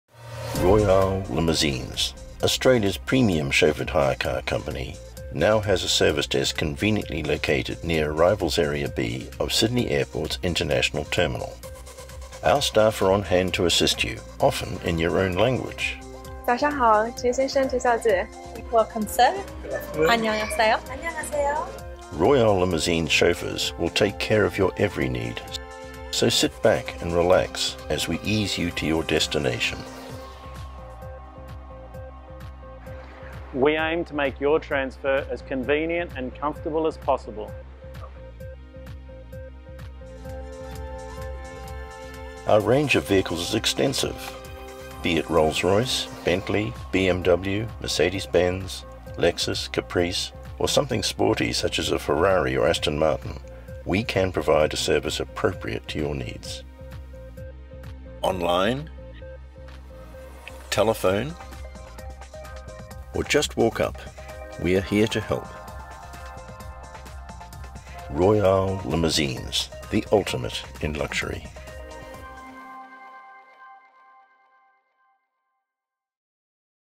Royale Limousines at Sydney Airport
VIP treatment for guests arriving at Sydney Airport. Our new Customer Service Desk located in the International Terminal Arrivals Hall B is a convenient way to organise your transfer upon arrival then proceed to Royale's exclusive parking area for a quick getaway.